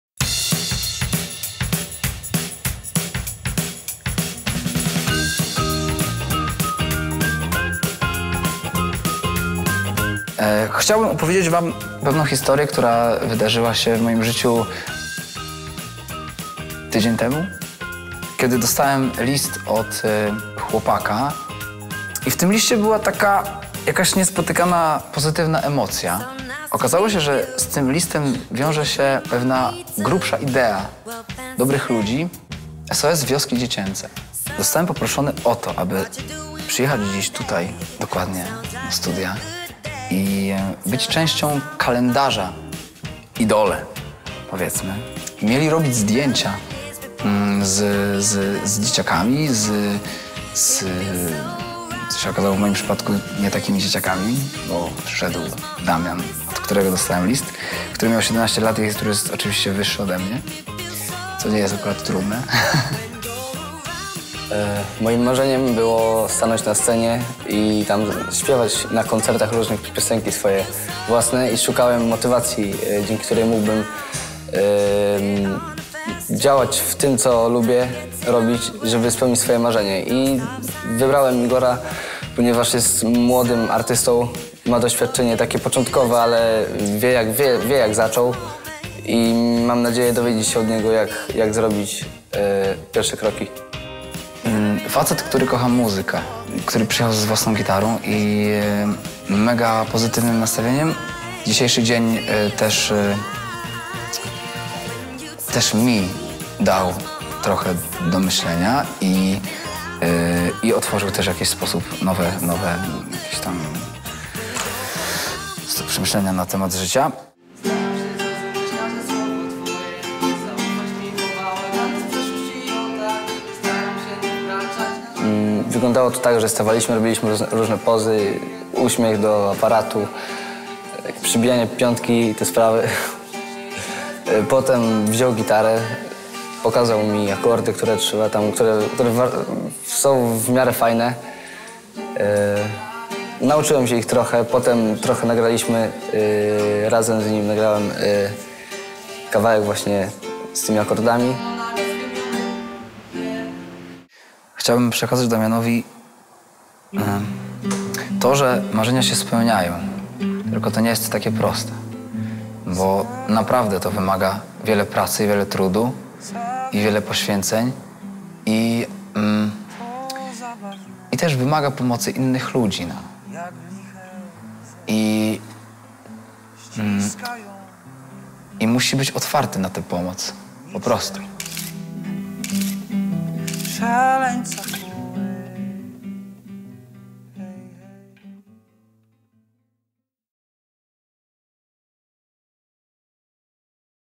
Igor Herbut i Damian, podopieczny Stowarzyszenia SOS Wioski Dziecięce w Polsce
Od 1 października w sklepach Kaufland oraz na stronie można kupić Kalendarz Charytatywny 2016.
W kalendarzu znajdziecie wyjątkowe zdjęcia dzieci (podopiecznych SOS Wiosek Dziecięcych) i ich idoli. Kupując kalendarz dajecie dzieciom szansę na lepszą przyszłość, bo zebrane fundusze zostaną przeznaczone na zajęcia wyrównawcze i rozwijające pasje podopiecznych Stowarzyszenia.

Pomóż nam stworzyć dom pełen miłości dla opuszczonych i osieroconych dzieci.